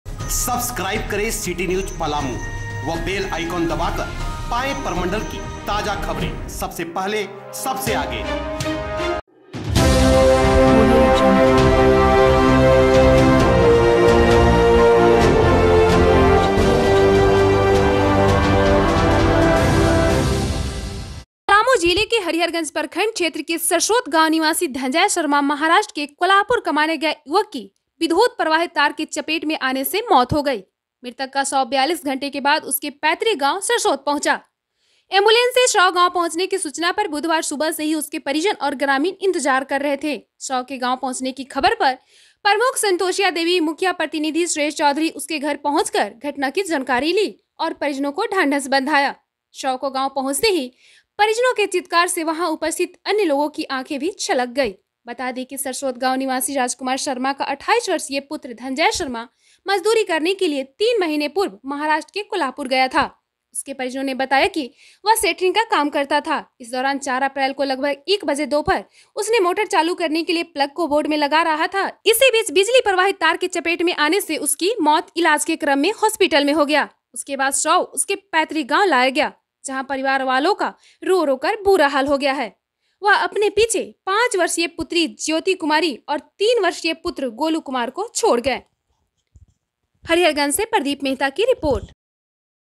पलामू - धनंजय शर्मा का शव गांव पहुंचते ही मचा कोहराम
हरिहरगंज से प्रदीप मेहता की रिपोर्ट